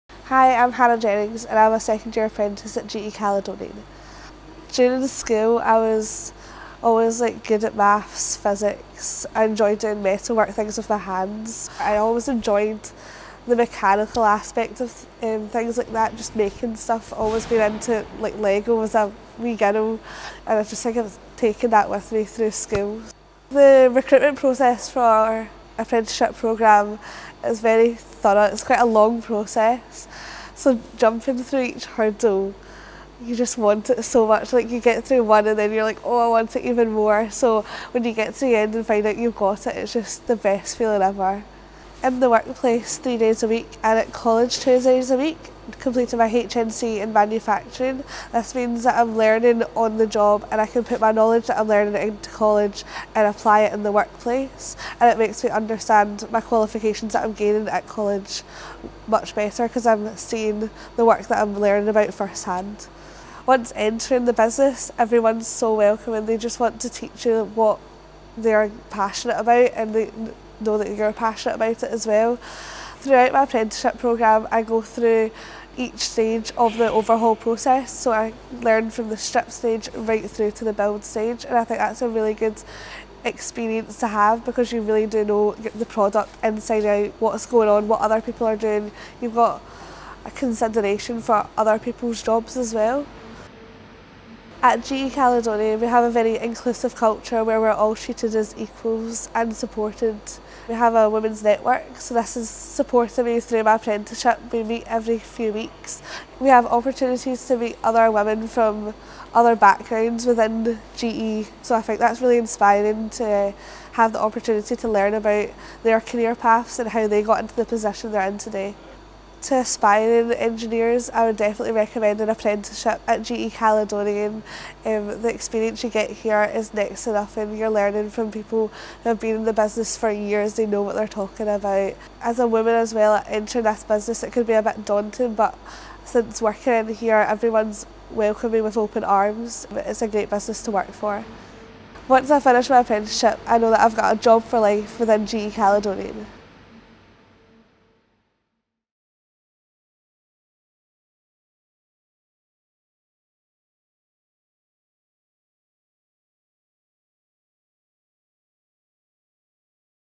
Meet Hannah Jennings
Meet Hannah! Hannah joined GE Caledonian in 2021 on a 3 year engineering apprenticeship programme and has since thrived as she continues to develop her skills in tandem with the aircraft engine overhaul process.

Hannah was inspired to join GE after members of our team visited her school in 2019. Hannah is an exceptional role model for all current and aspiring apprentices and we are so proud to have her on our team.

to view open roles and see how your career can take flight!